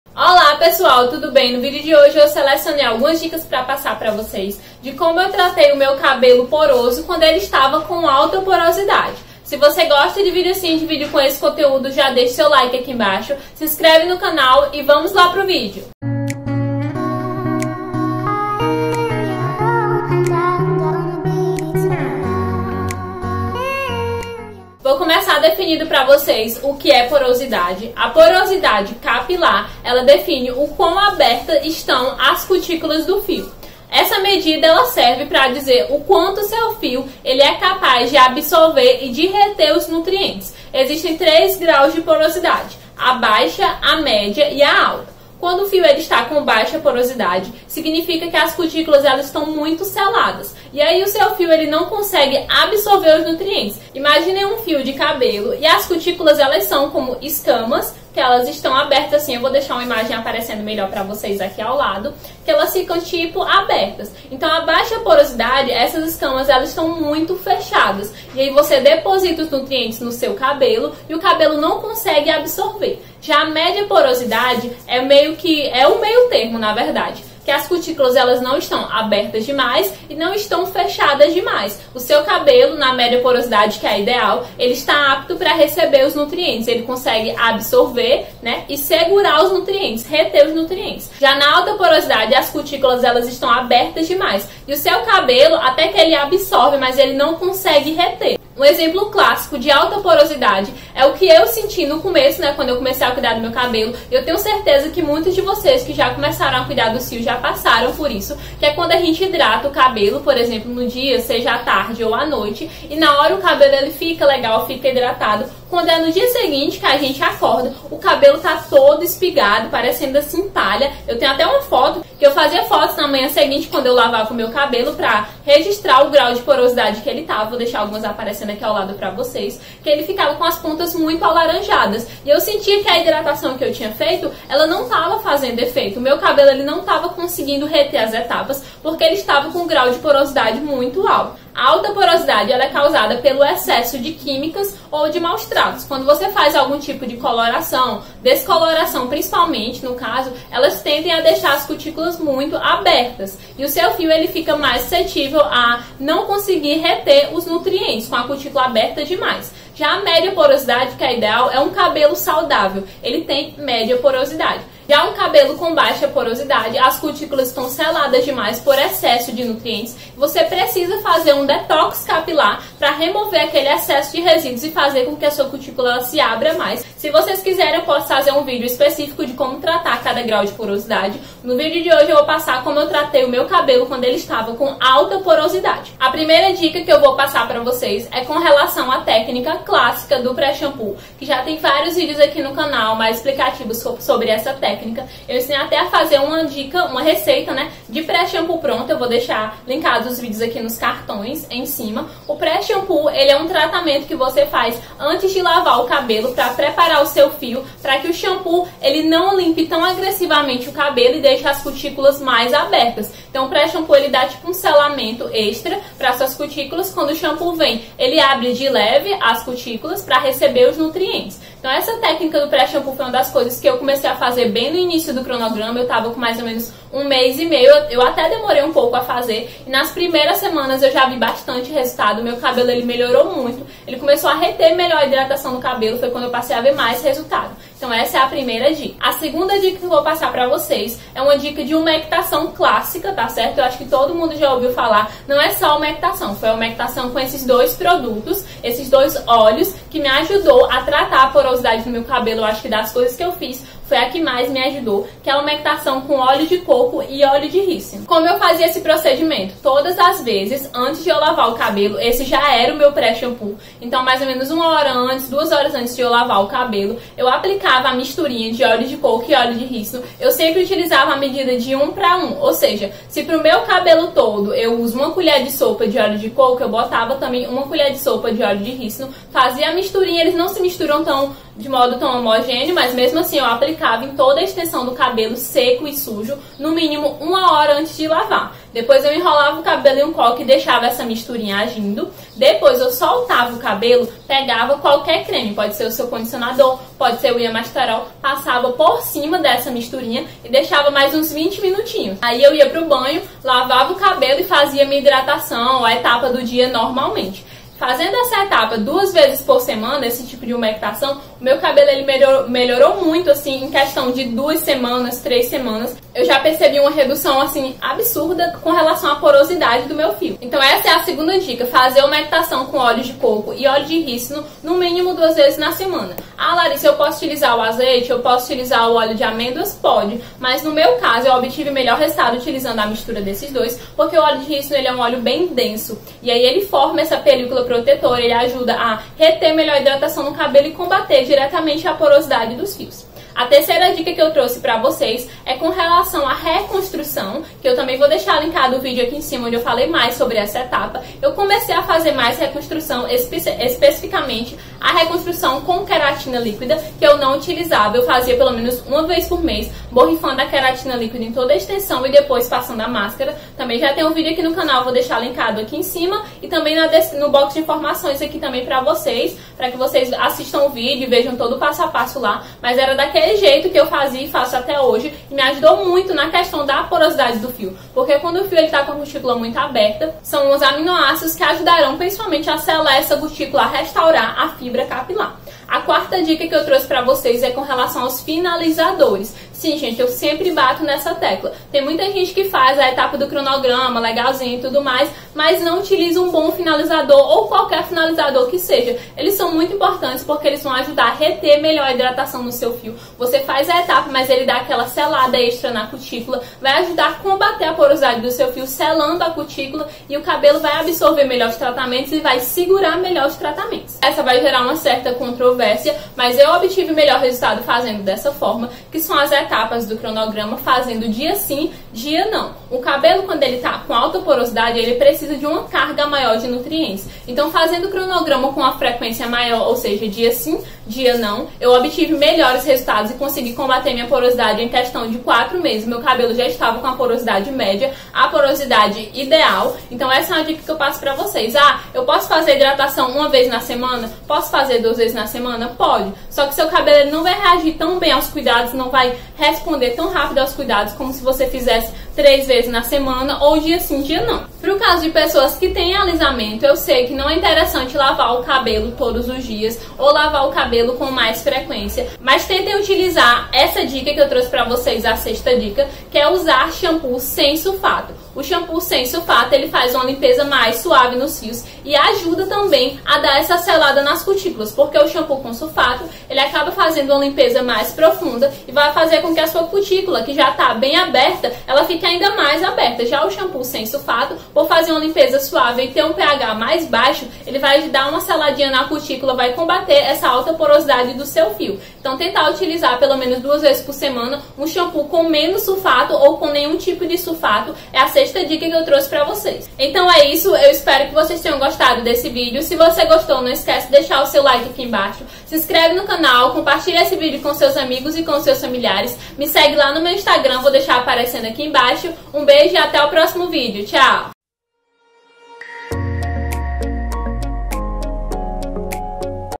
⚠️VOCÊ SABE TRATAR UM CABELO COM ALTA POROSIDADE?! - DICAS P/ TRATAR CABELO POROSO ✔️| Larisse Gama
Fiz esse vídeo com muito carinho e nele eu listei as 6 principais dicas que me ajudaram a tratar meu cabelo quando ele estava com alta porosidade.
Espero do fundo do coração que vocês gostem e que com esse vídeo eu possa ter ajudado vocês! Fiquem ligadinhos que logo teremos mais vídeos!

Deixem suas dúvidas nos comentários, será um prazer respondê-las!

Beijo grande!